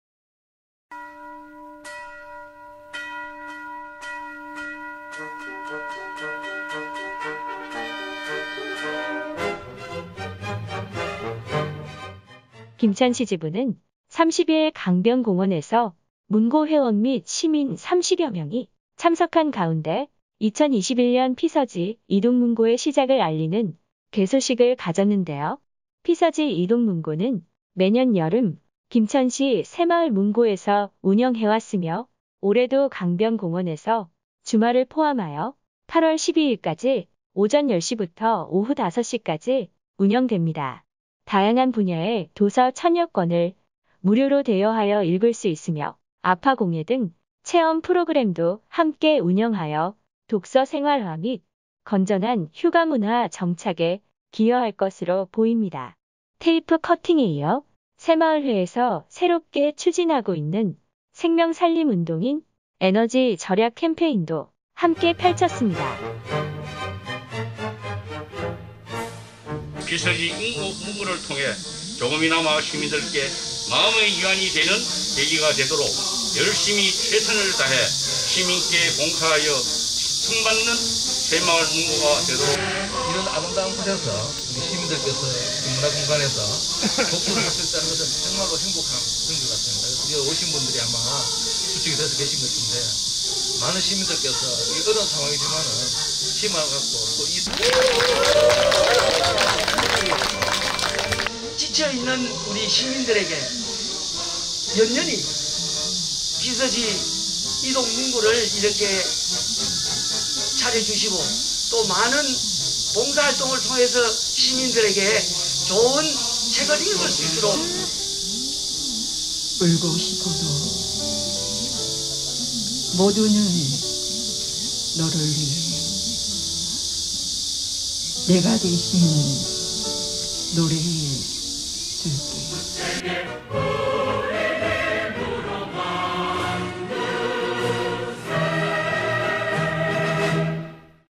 도심속 공원 피서지 이동문고
새마을문고 김천시 지부 강변공원에서 '2021년 피서지 이동문고' 개소식